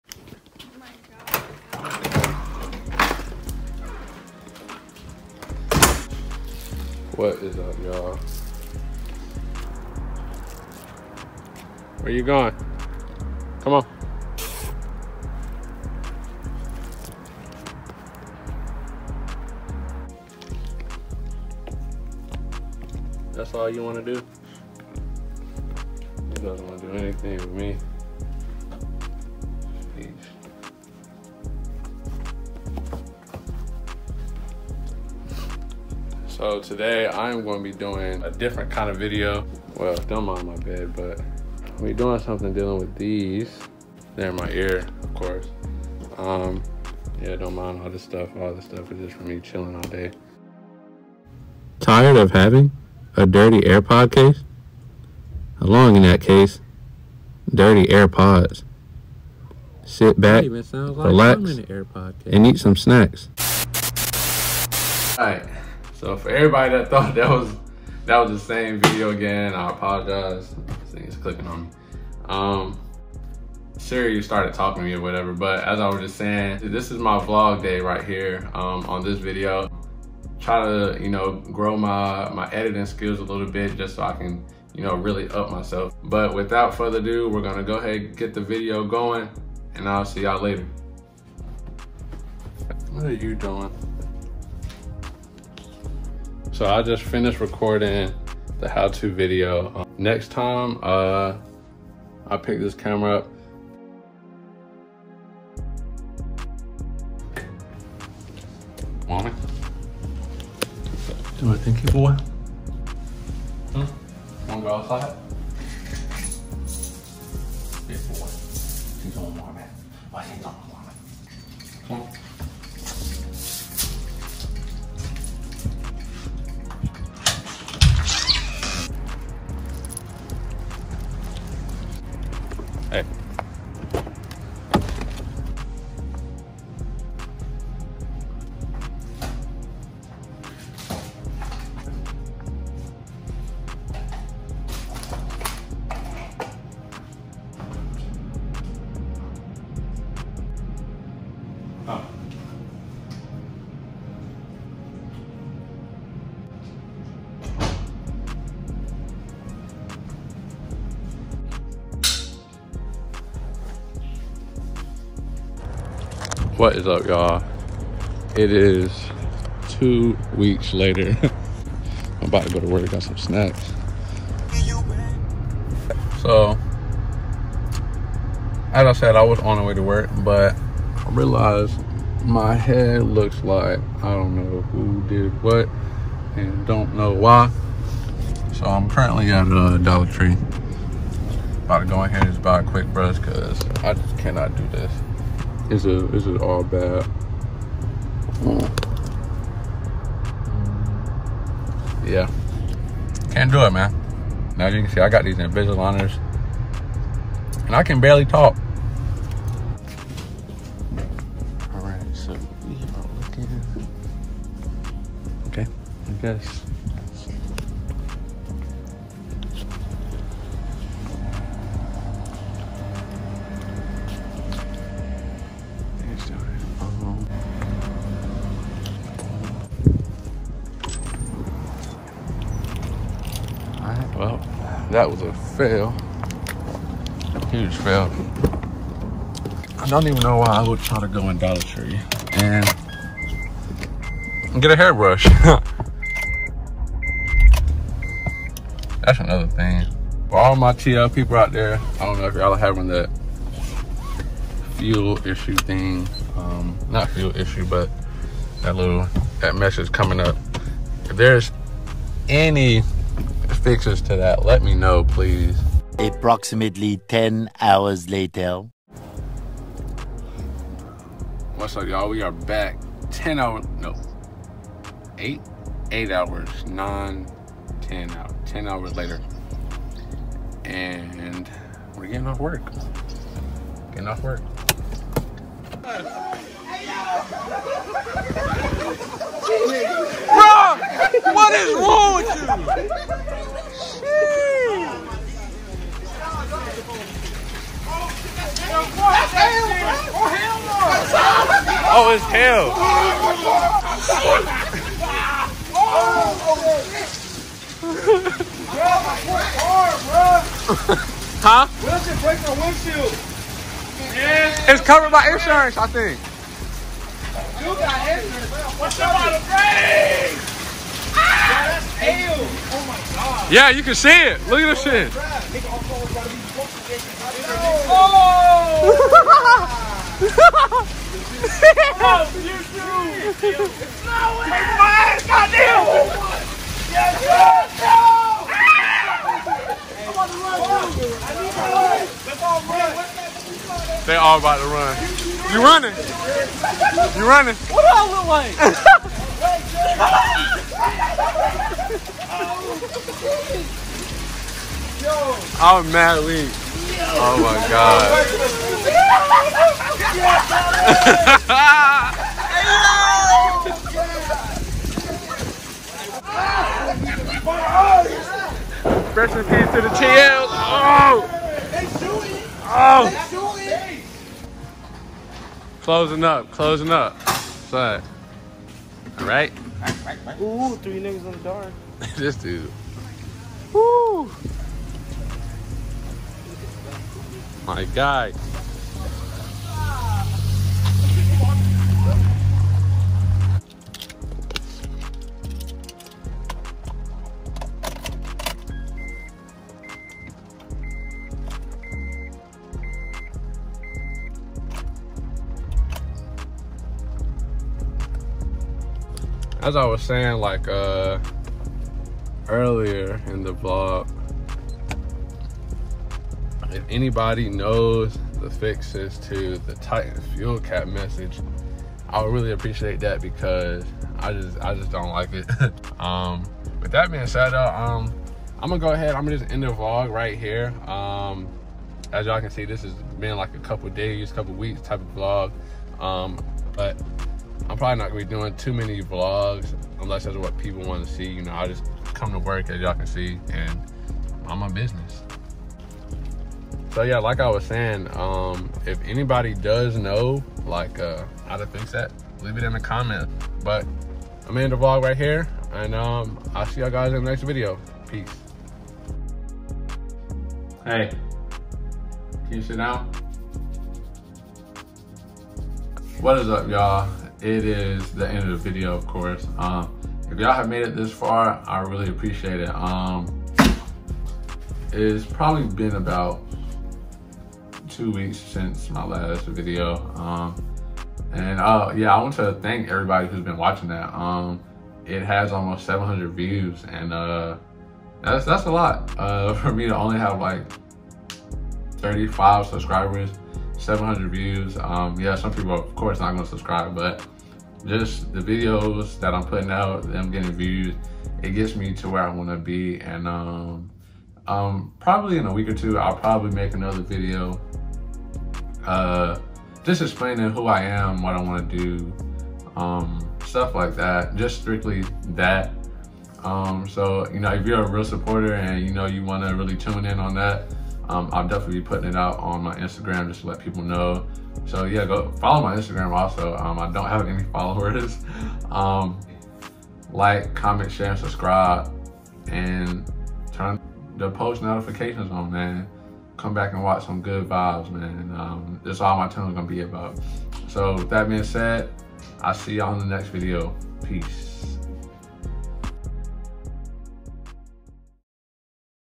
Rip To The TL’s | One Month Later ...
What’s up y’all! In today’s video we have a few things going on ... Unbrusbed head, hail storm, and the dog that tore up my car! Tune in on my channel for more uploads and ACURA TL TYPE S content!!! See y’all later 🤘🏾